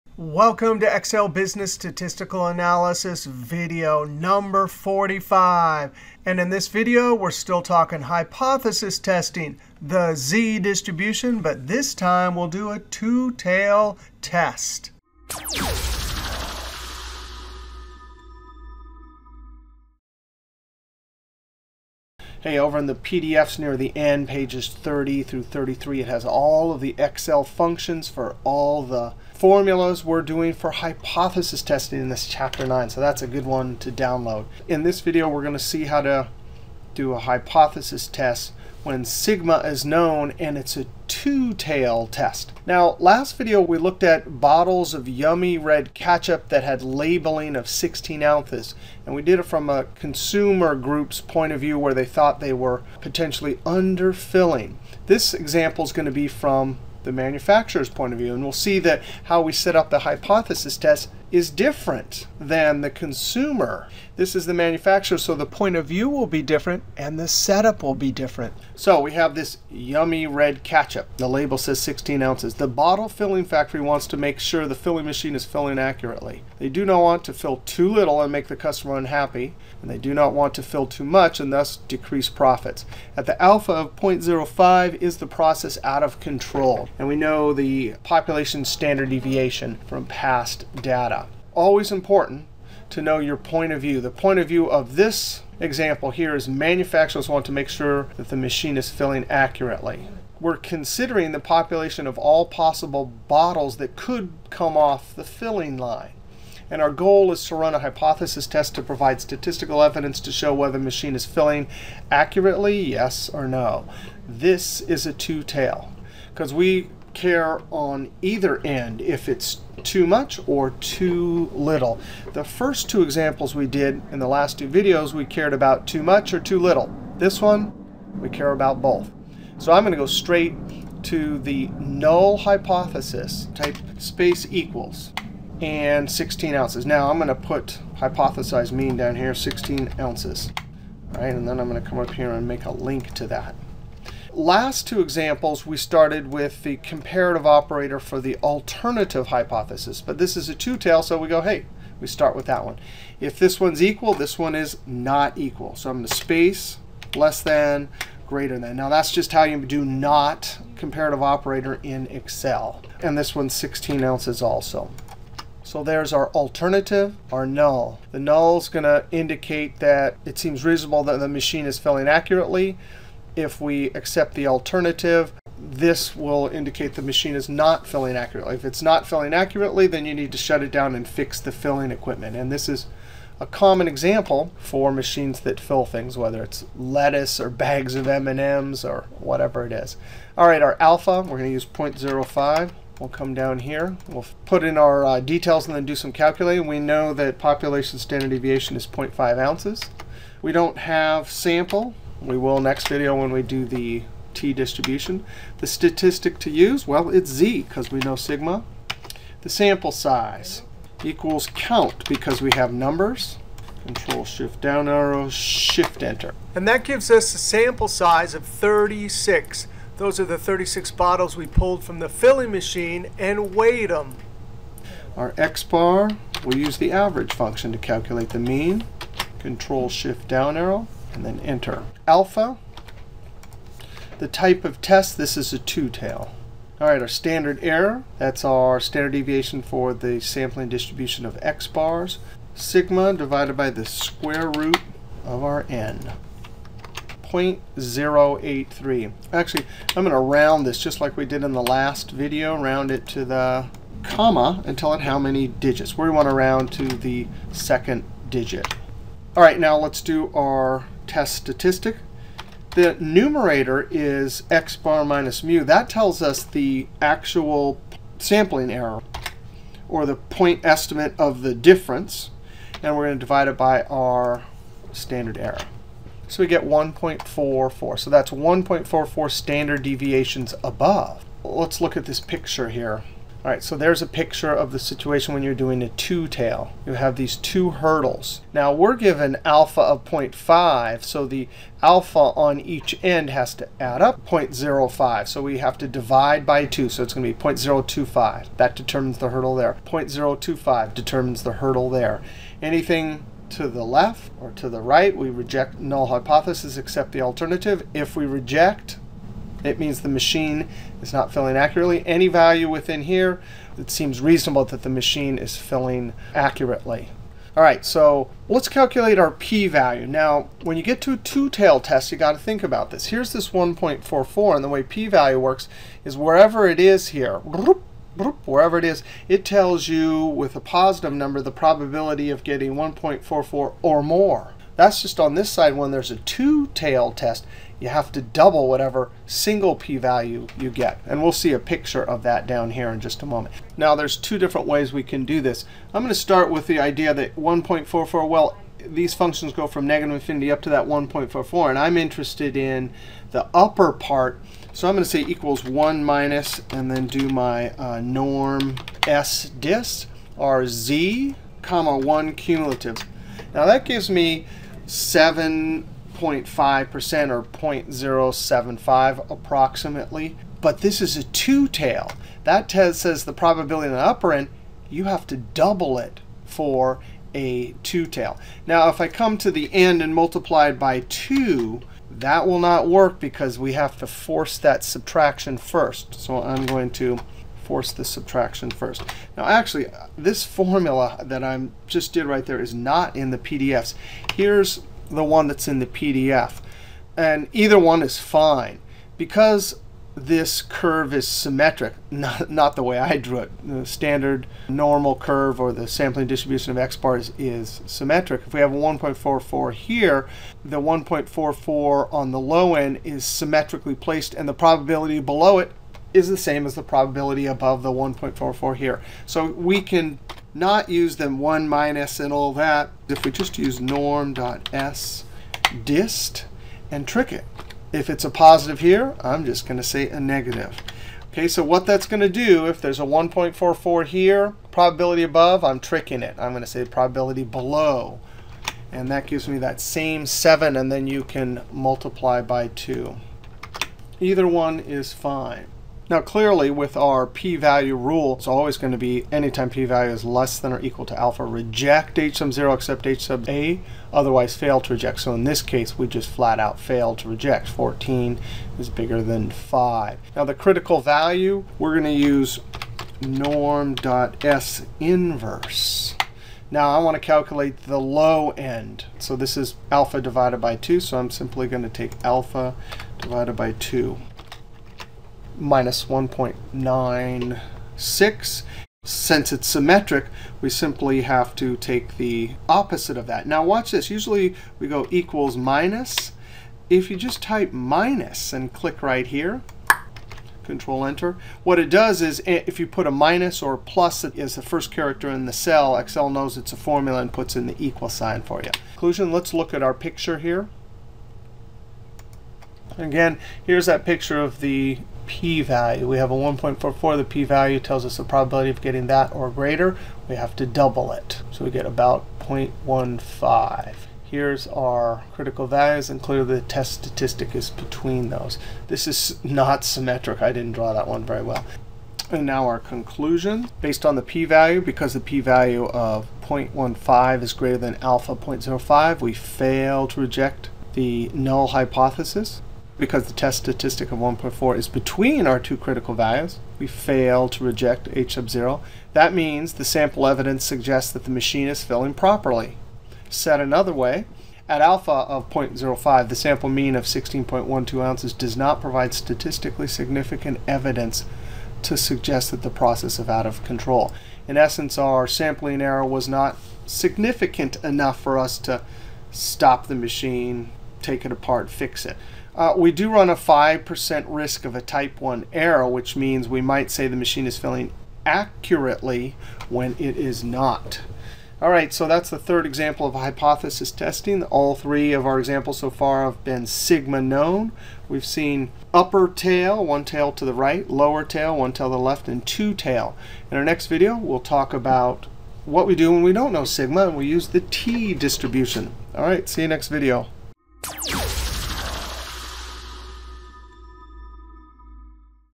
Excel Statistical Analysis 45: Hypothesis Testing with Z Distribution, Two Tail Test Example
Learn about Two Tail Hypothesis Test with z Distribution. Learn about the Excel worksheet functions NORM.S.DIST and NORM.S.INV

Topics:
1. (00:00) Introduction
2. (00:22) Two Tail Test with z Distribution from point of view of a manufacturer concerned with filling machine accuracy
3. (05:17) Calculate p-value using the NORM.S.DIST function
4. (07:39) Calculate critical value using the NORM.S.INV function
5. (09:31) Summary of video
6. (09:43) Closing, Next Video and Video Links